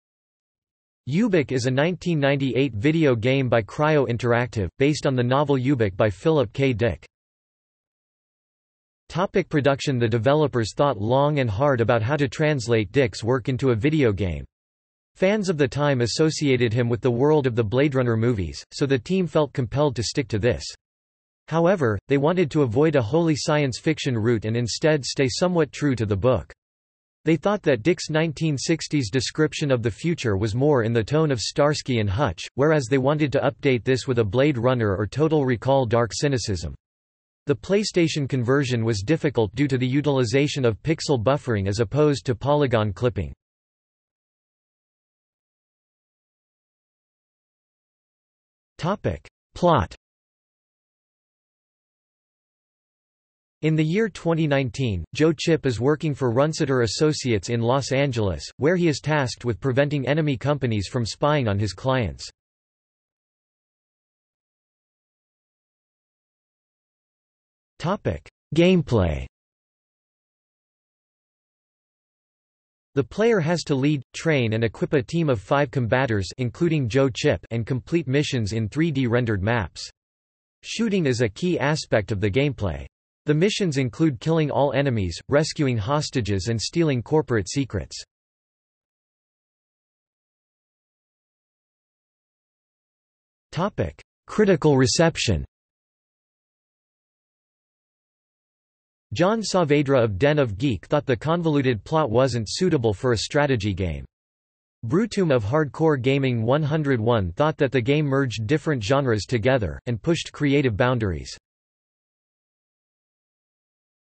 Ubik (video game) | Wikipedia audio article
00:00:08 1 Production
00:00:45 2 Plot
00:01:04 3 Gameplay
00:01:29 4 Critical reception

- Socrates

SUMMARY
Ubik is a 1998 video game by Cryo Interactive, based on the novel UBIK by Philip K. Dick.